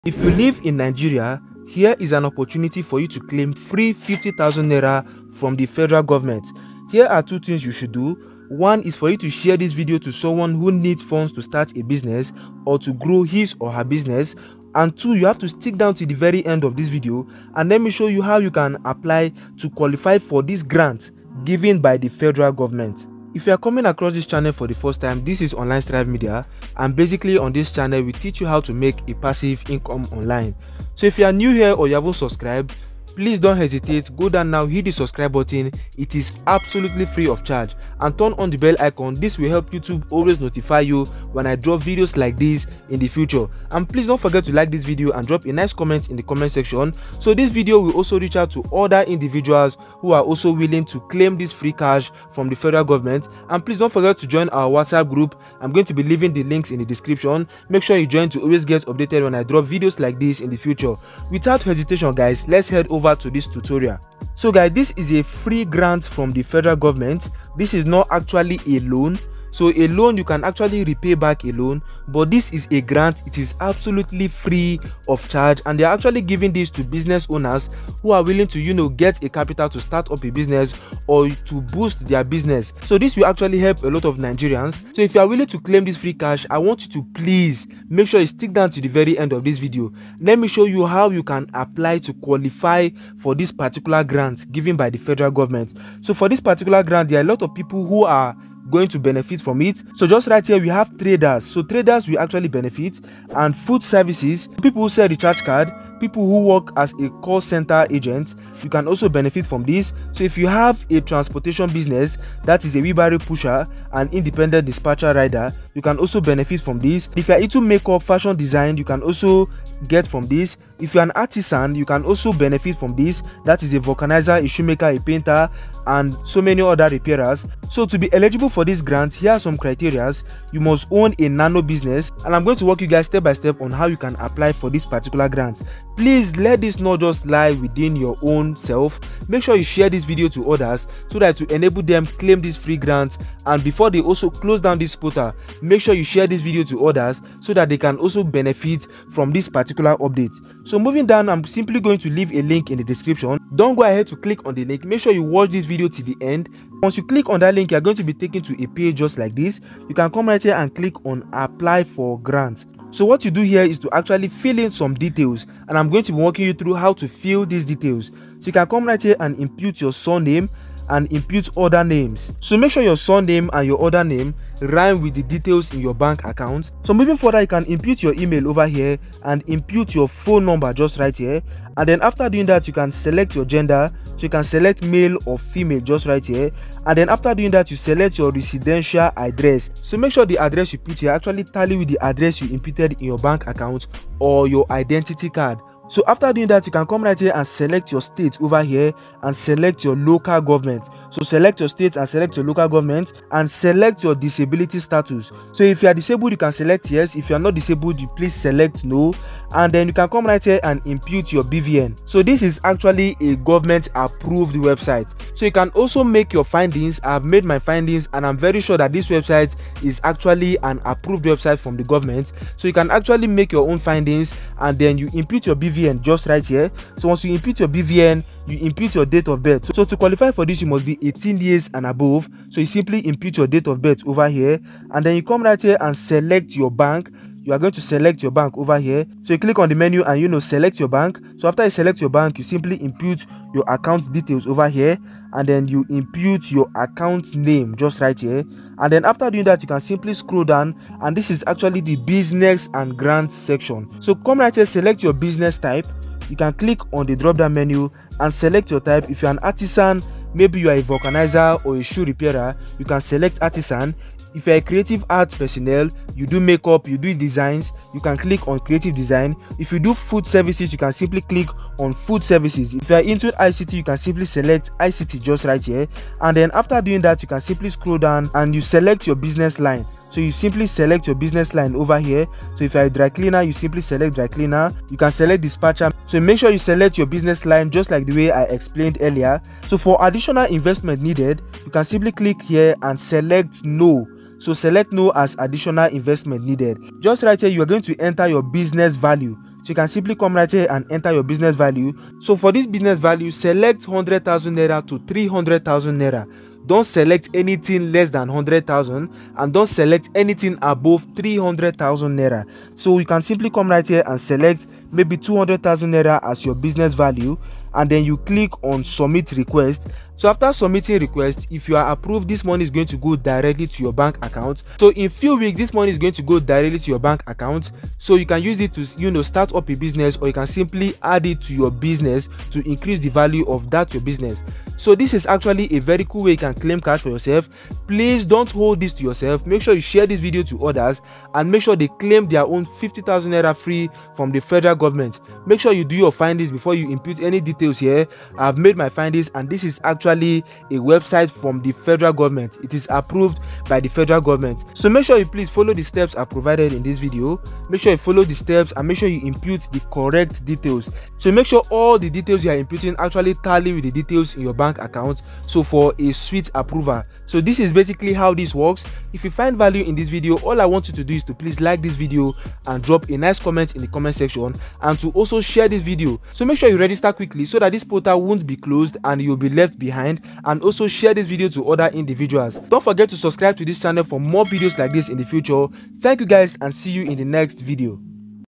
GET FREE ₦50000 grant in Nigeria (APPLY NOW) || make money online in Nigeria
If you live in Nigeria Here is an opportunity for you to claim free 50000 naira grant from the federal government to start up a business for yourself.
Watch this video till the end to get every details about this grant and also how to apply and get approved.